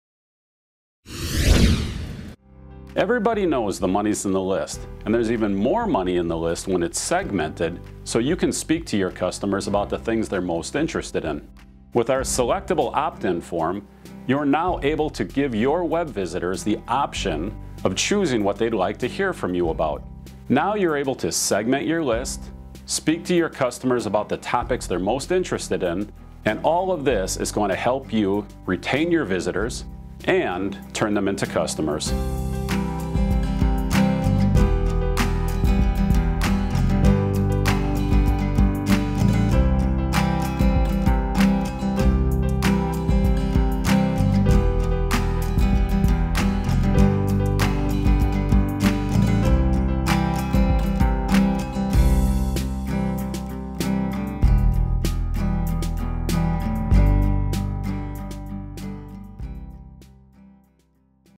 Selectable CRM / Autoresponder Opt-in Forms
- CRM segmenting is the "magic sauce" for small business online marketing and the sooner you start the segmenting process, the better your business will operate.

Using a "Selectable Opt-In Form" starts the CRM list segmenting at the very first interaction with your prospects and customers.